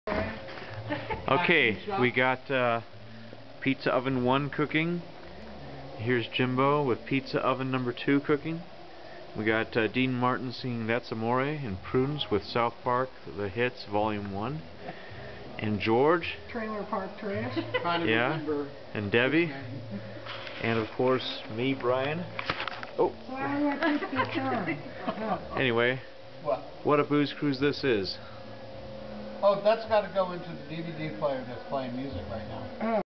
Booze Cruise 2007
A short video from the pizza cooking on May 4, 2007, at Jimbo's Booze Cruise at Pinyon Pines.

Brian Nomi